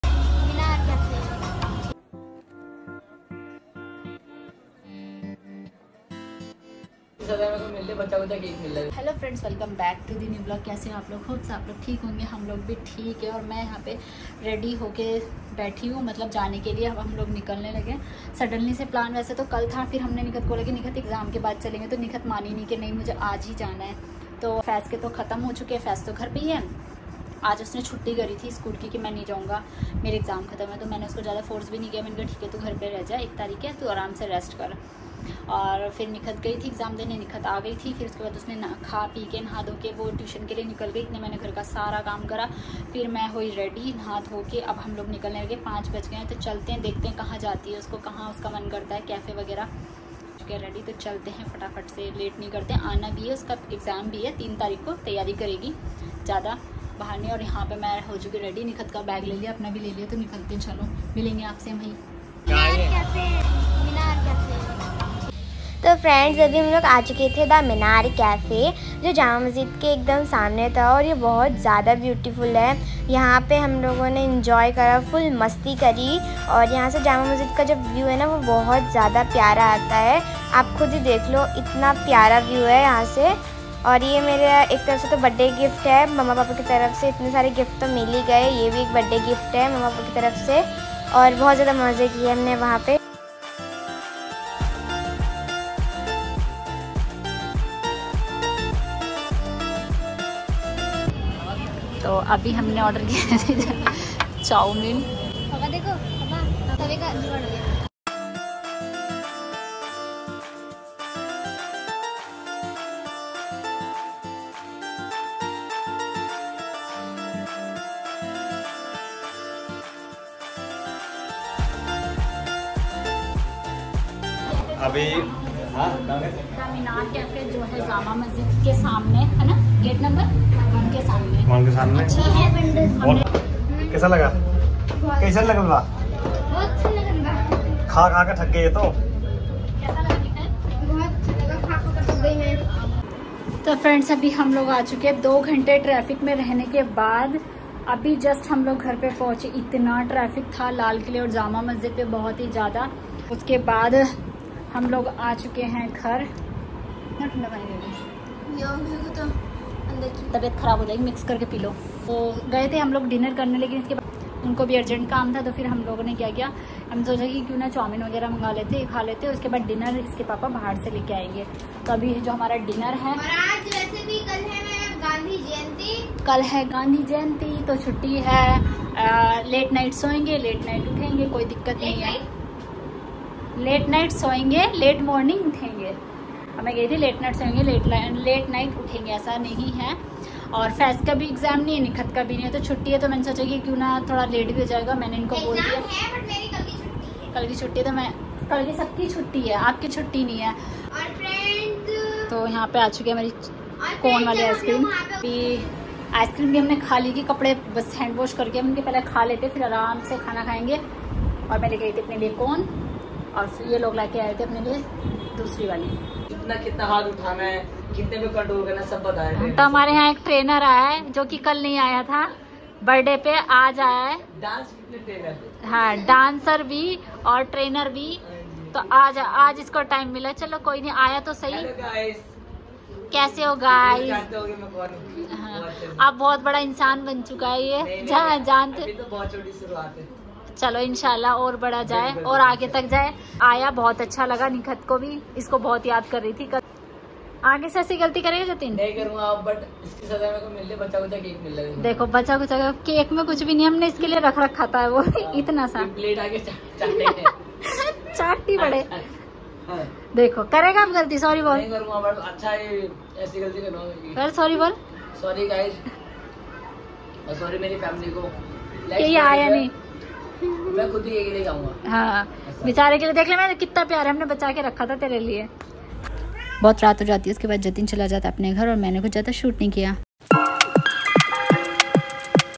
AAJ GAYE PHIR SE GHUMNE |OR GHAR PR AAJ JATIN MILNE AAYA
Jama masjid
The minar cafe
Café
Delhi Jama Masjid
Hotel